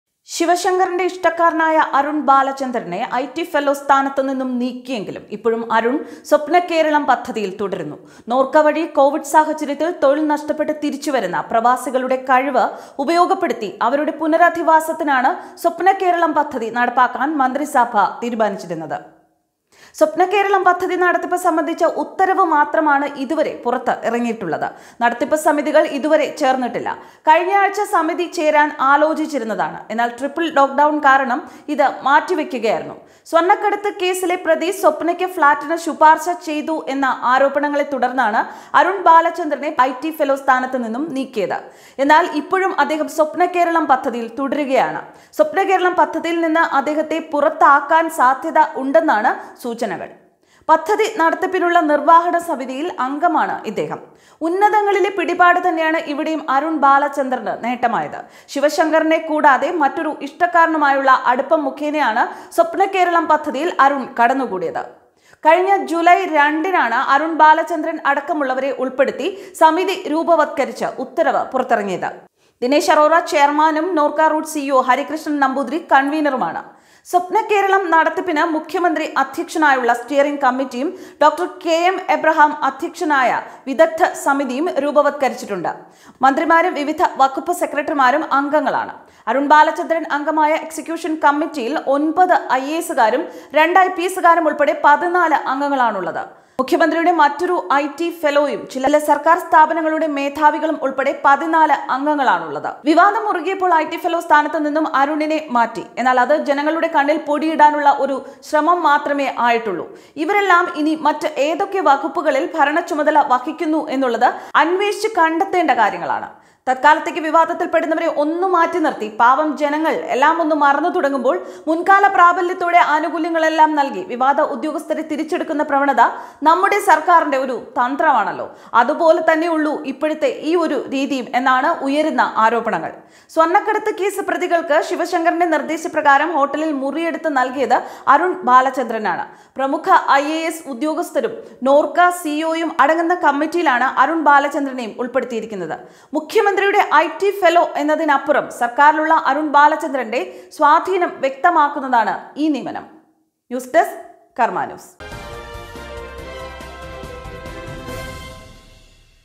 അരുണിനെ പുറത്താക്കാൻ മനസ് വരാതെ സർക്കാർ | karma news
IT ഫെല്ലോ സ്ഥാനതു നിന്ന് പുറത്താക്കി, എന്നാൽ രഹസ്യമായി സ്വപ്നകേരളത്തിൽ അരുൺ ബാലചന്ദ്രന്  പദവി.